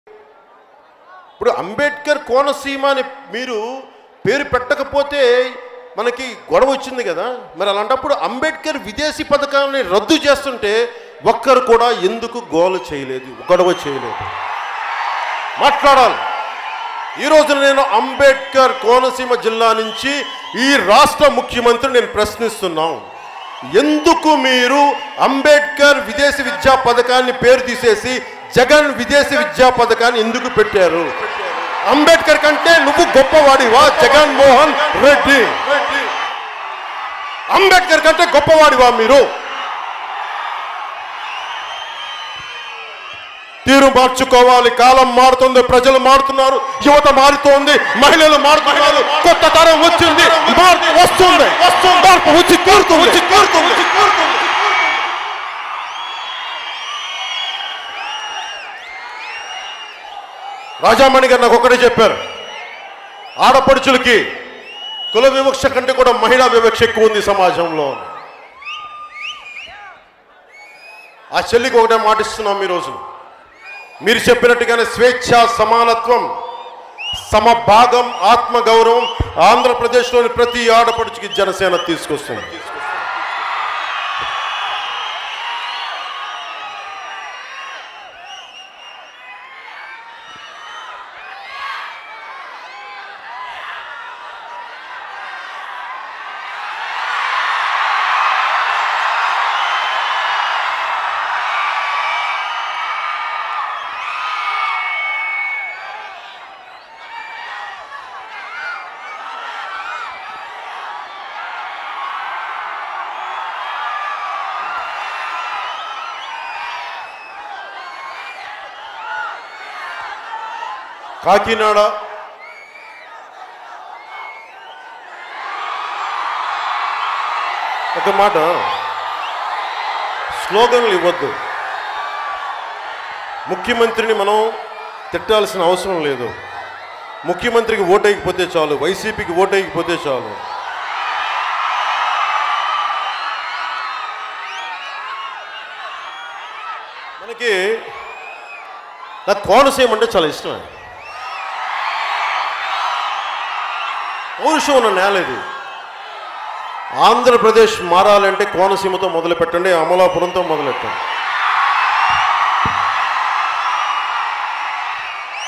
పాపం జగన్ ని ఏడిపించకండిరా 😂 | Pawan Kalyan Satires On YS Jagan Over His Slogans At Amalapuram | Stv
Watch here పాపం జగన్ ని ఏడిపించకండిరా 😂 | Pawan Kalyan Satires On YS Jagan Over His Slogans At Amalapuram

ఇది దమ్మున్న మేనిఫెస్టో అంటే👌 | Pawan Kalyan Great Shanmukha Vyuha Manifesto From Janasena